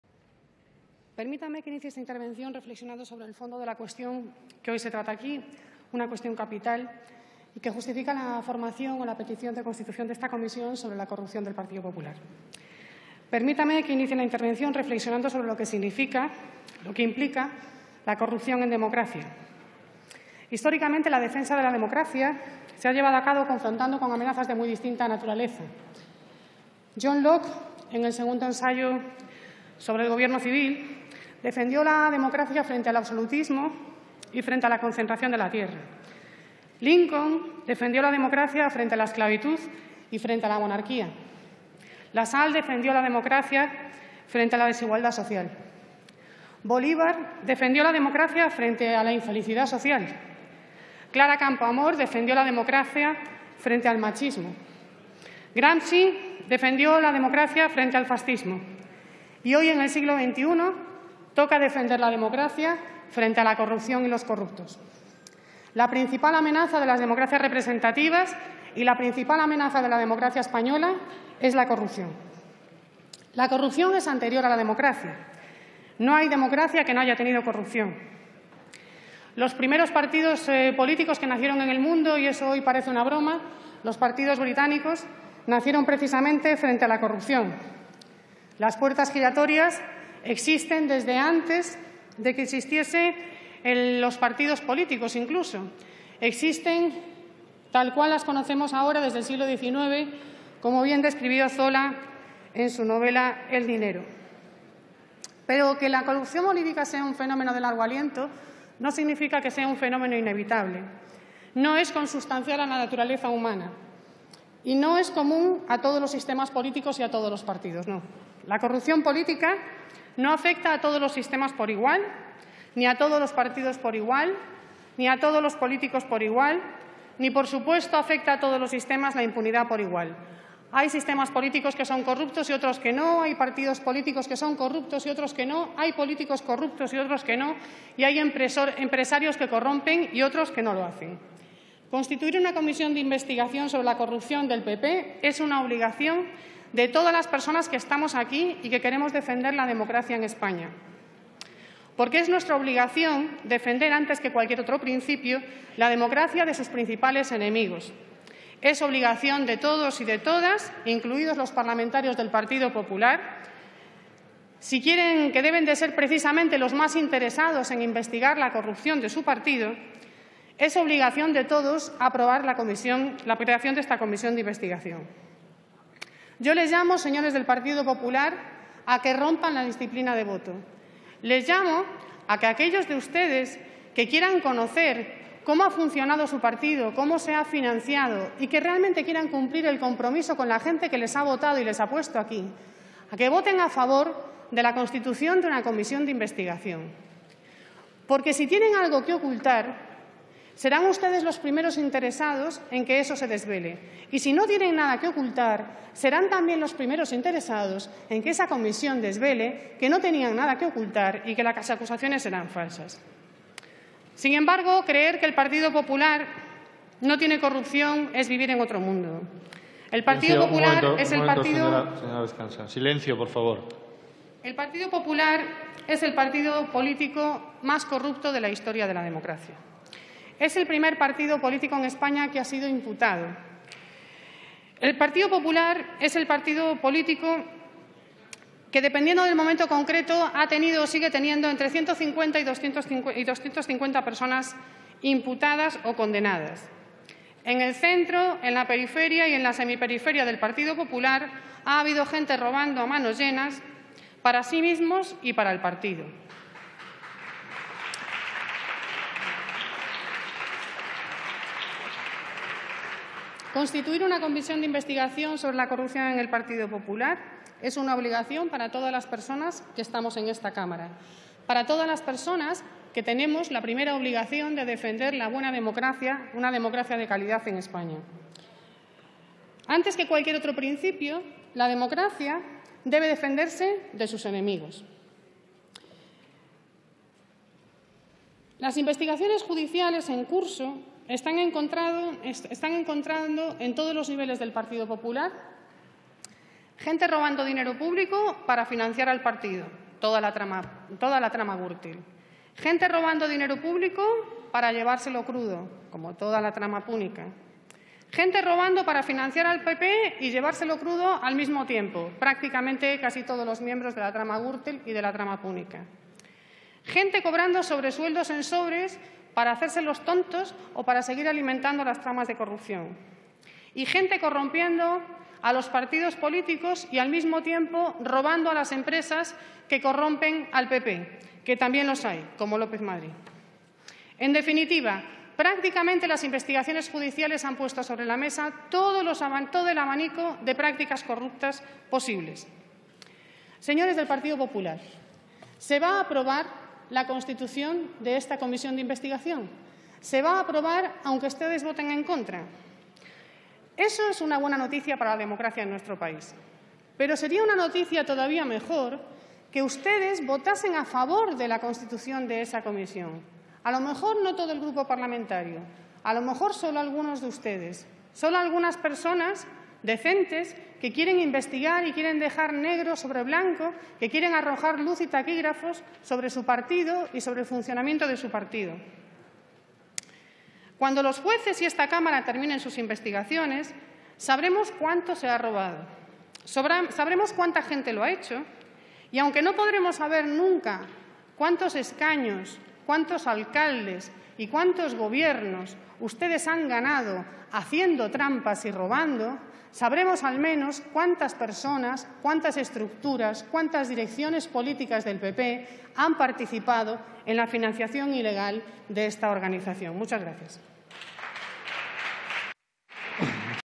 Carolina Bescansa: "Es nuestra obligación defender la democracia de sus enemigos"
Sobre la solicitud de creación de una Comisión de Investigación relativa a la presunta financiación ilegal del Partido Popular.